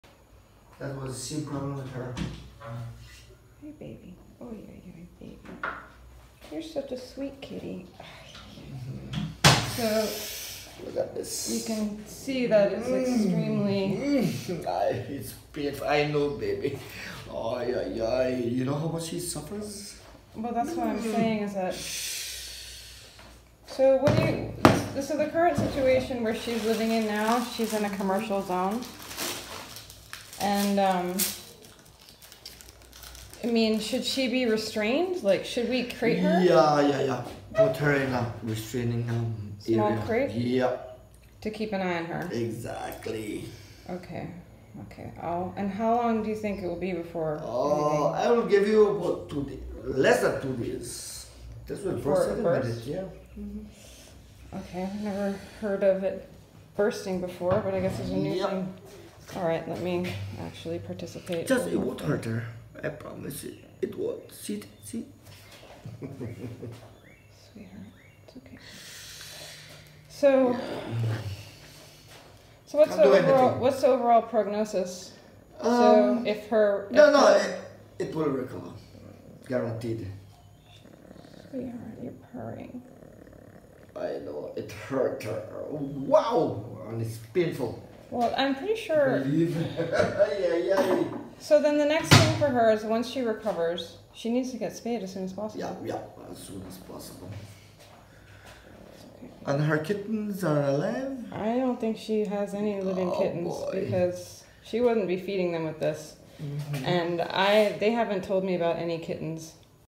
Terrible case of Mastisis in a young community cat from Belize
Mastitis is a bacterial infection in the milk ducts of female cats which causes the mammary gland to become blocked, thus milk cannot be released. The teat swells and becomes red and painful to the touch. Sometimes pus or discolored milk may be released. Often kittens are abandoned and starve to death if no intervention occurs.  Spaying female kittens before their first heat can prevent this unnecessary suffering!